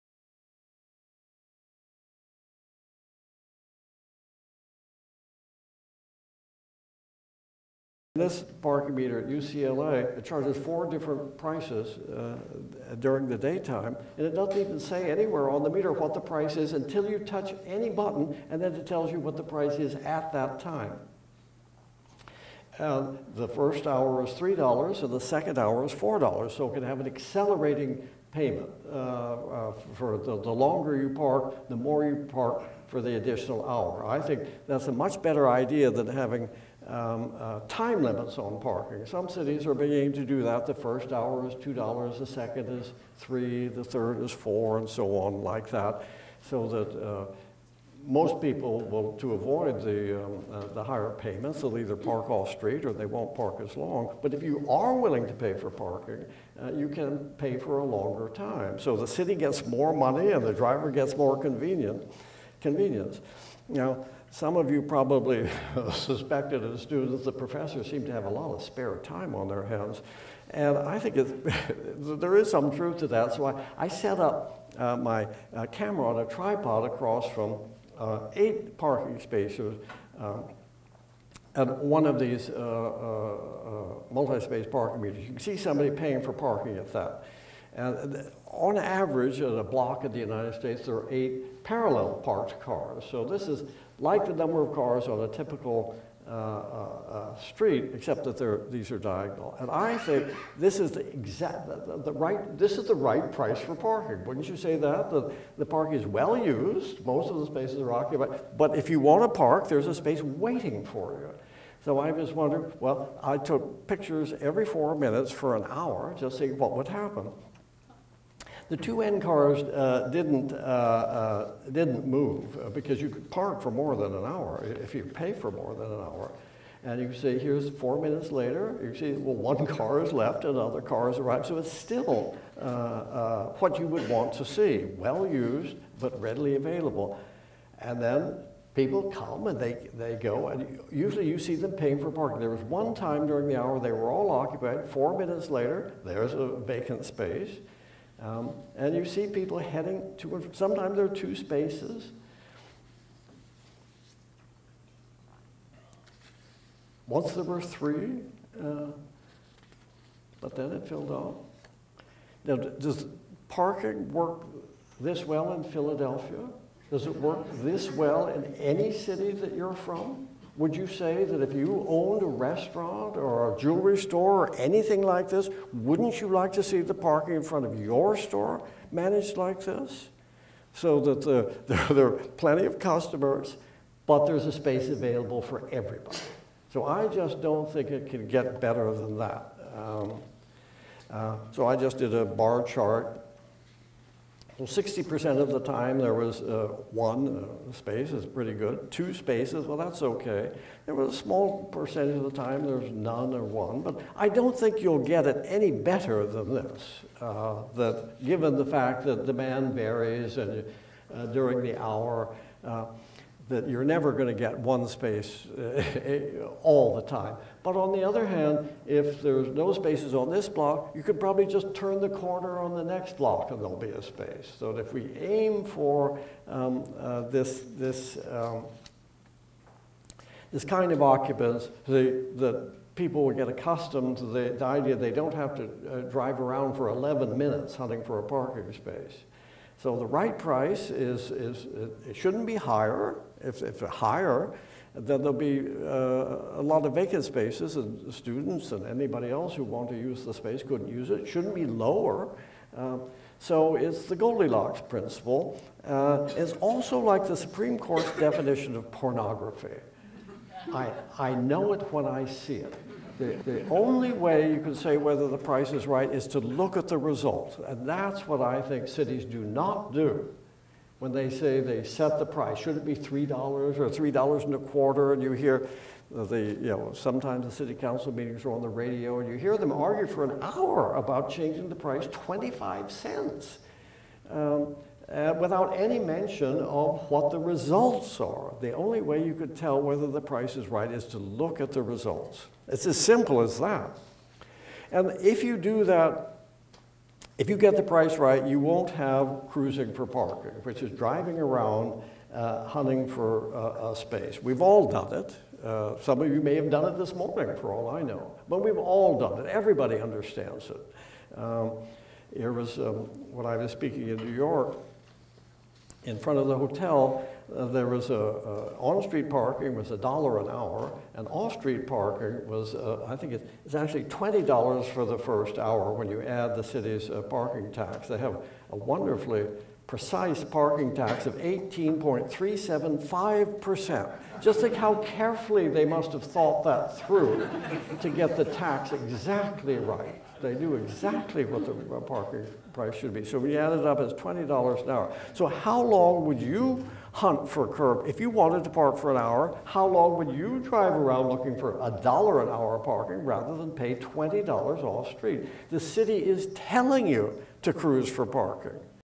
Donald Shoup: This is the right price of parking
Parking expert Donald Shoup addressing the Delaware Valley Smart Growth Alliance Spring Forum on May 5, 2016 in Philadelphia.

In this clip, he describes how parking works at UCLA, and says they have figured out the "right price" of parking.